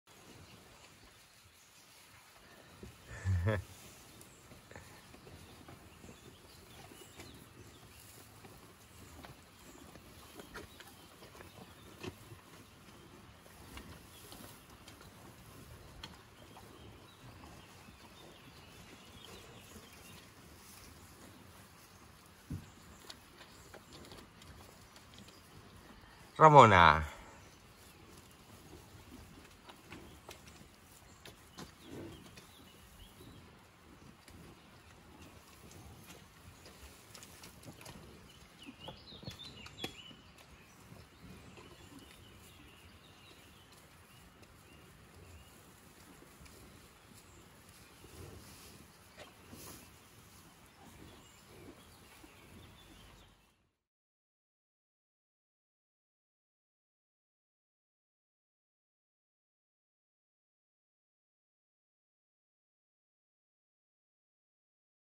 Ramona, Uma, Nayara y Araitz | La nueva vida de 3 vacas y 1 ternera rescatadas
Pues parece que ya todos son una familia feliz: Ramona, Uma, Nayara, Araitz, Savi y Cristian 😊

Desde que llegaron las vacas de Navarra, están viviendo en la zona de los terneros Savi y Cristian. Al principio tuvieron algunos roces, pero ya están limados y ahora se llevan todos estupendamente.

Como podéis ver en el vídeo desayunan todos juntitios y bien ordenados. Aunque sí se nota quien manda, que es Ramona, que no duda en imponerse a los demás para llevarse el mejor bocado, vamos lo que sucede en todas las familias 😂